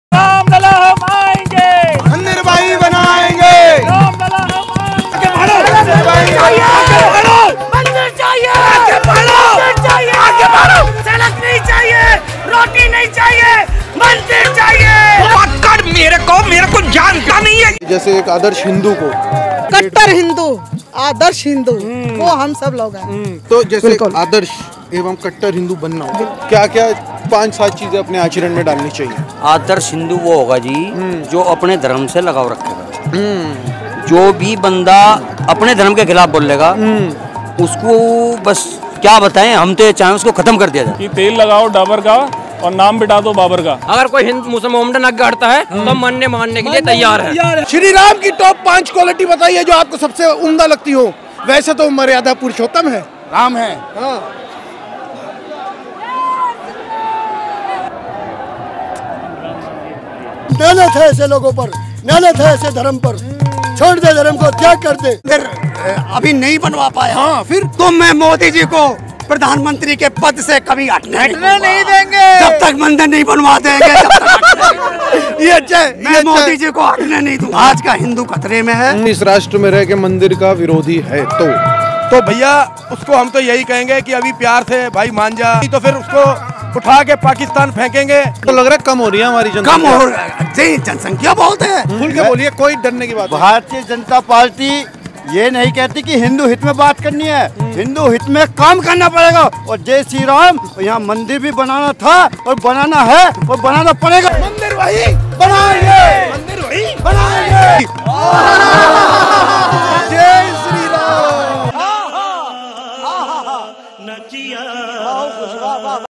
Indian people on Ram mandir | How to become good Hindu | ft. Scoopwhoop Unscripted | Roastist
In this video there is a road survey of indians on ram mandir formation
in Ayodhya done by Youtube channel scoopwhoop unscripted

Credits:  Source ( Scoopwhoop Unscripted)
Obtained From ( Their Youtube Channel )